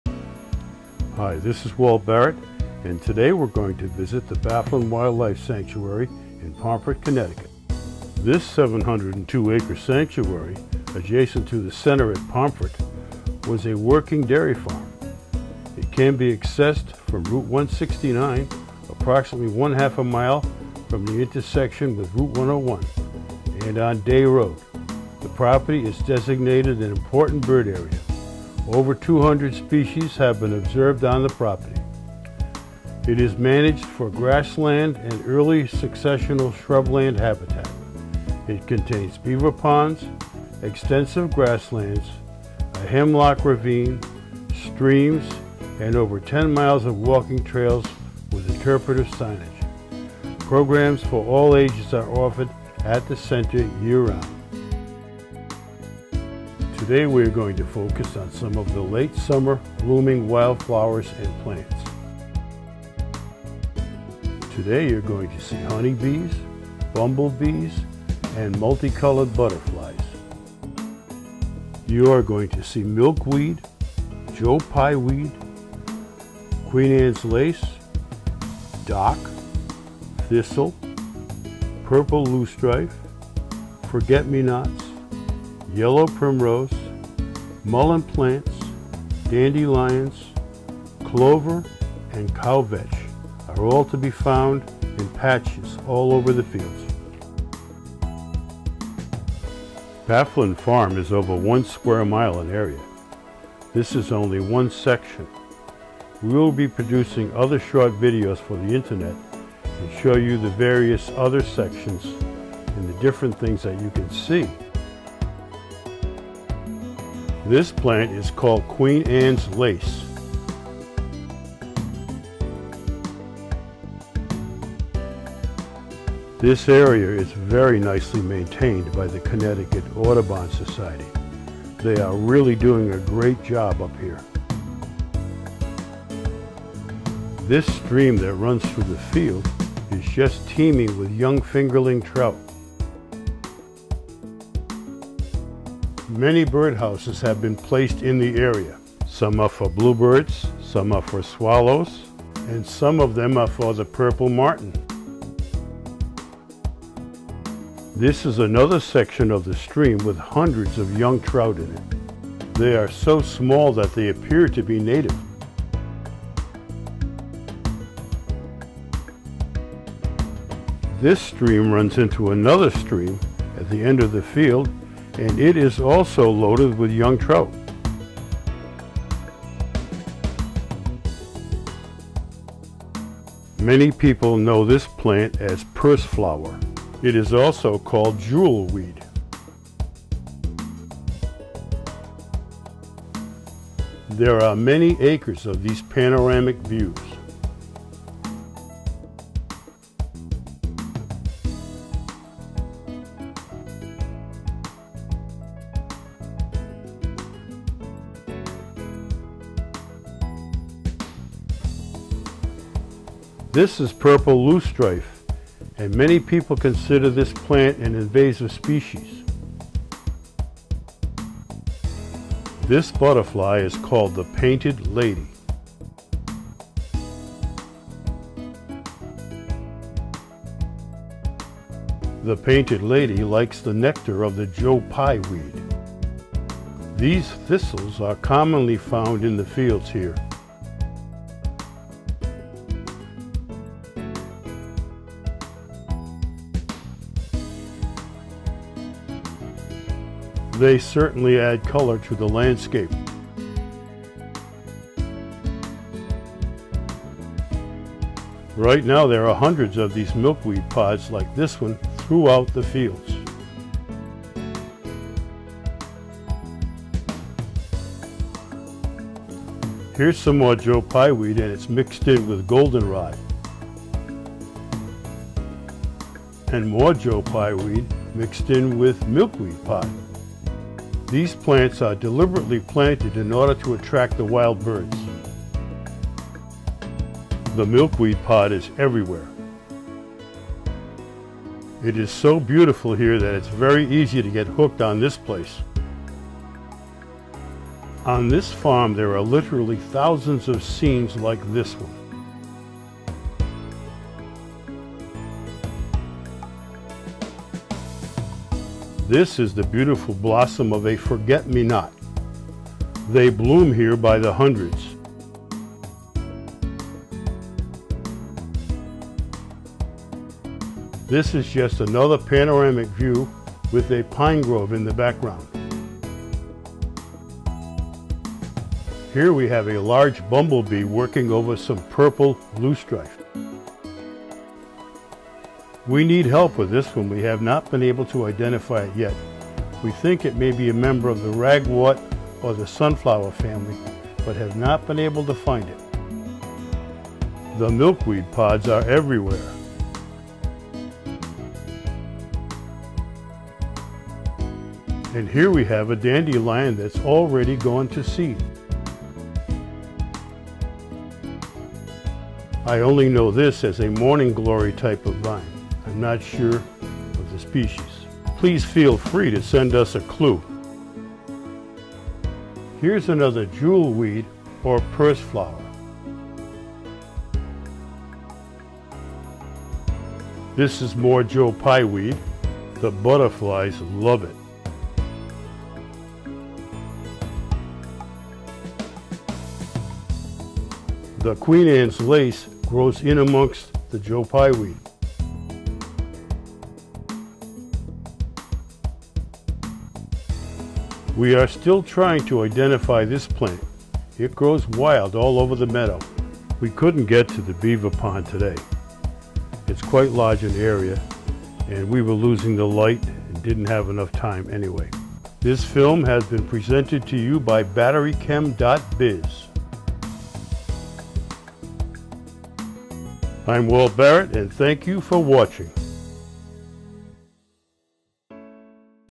Bafflin Wildlife Sanctuary Pomfret CT by Walt Barrett
A great showing of late summer flowers. A Walt Barrett Film Narrated by Walt Barrett